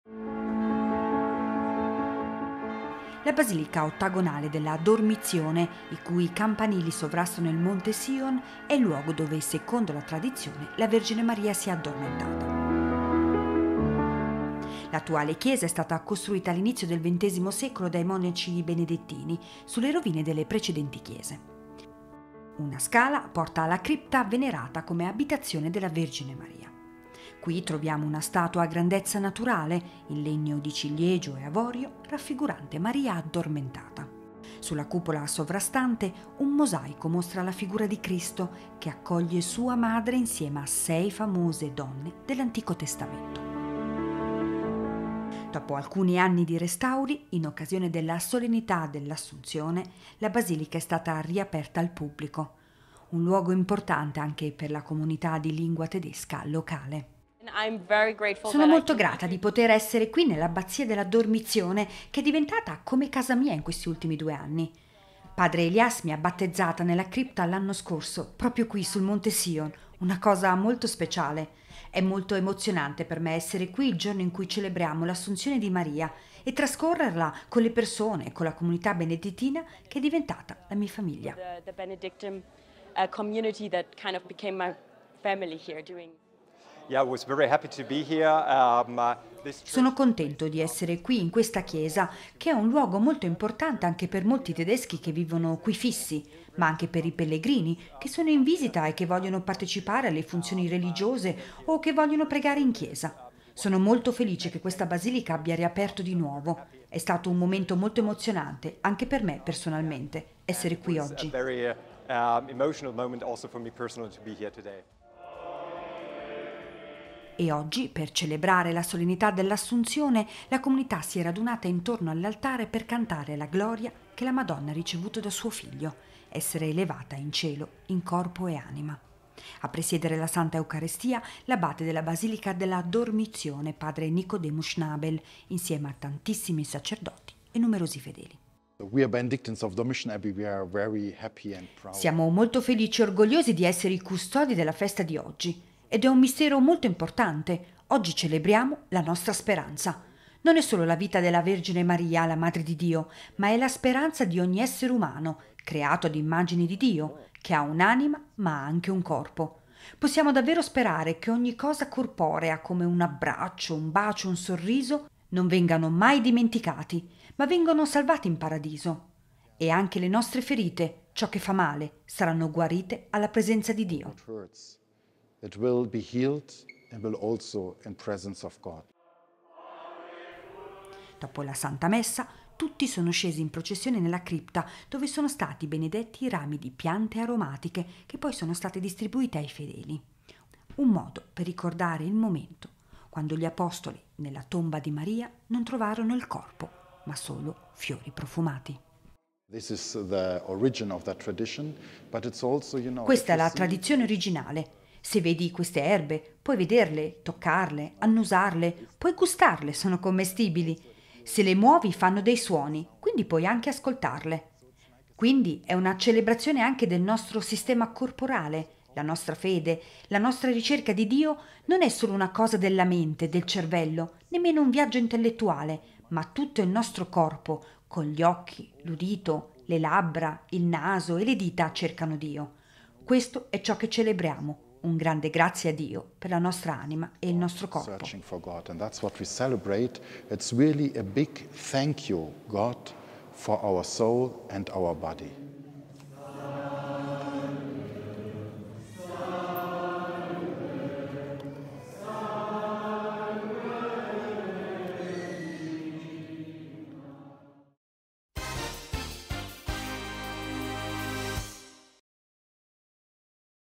Basilica della Dormizione: Maria che onora Cristo in corpo e anima
Più: Nel giorno della festa dell’Assunta, alla Basilica della Dormizione, situata nel monte Sion, alla scoperta del luogo dove secondo la tradizione la Vergine Maria si è addormentata.
Ed oggi per celebrare la Solennità dell’Assunzione, la comunità si è radunata intorno all’altare per cantare la gloria che la Madonna ha ricevuto da suo Figlio: essere elevata in cielo in corpo e anima.
A presiedere la Santa Eucaristia, l’abate Padre Nikodemus Schanabel, insieme a tantissimi sacerdoti e numerosi fedeli.